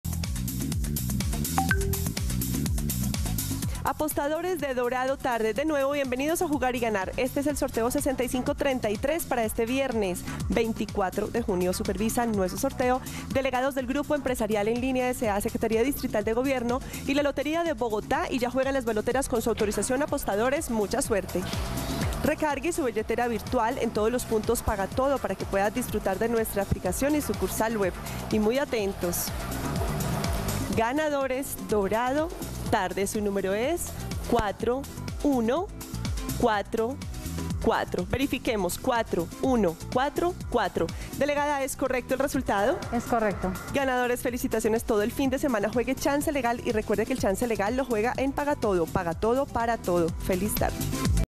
EL DORADO TARDE Resultado del VIERNES 24 DE JUNIO de 2022 ✅🥇🔥💰
Hola buenas tardes, te presentamos los resultados de las loterías y chances en Colombia, vamos con el Resultados del Chance Dorado tarde del VIERNES 24 DE JUNIO de 2022.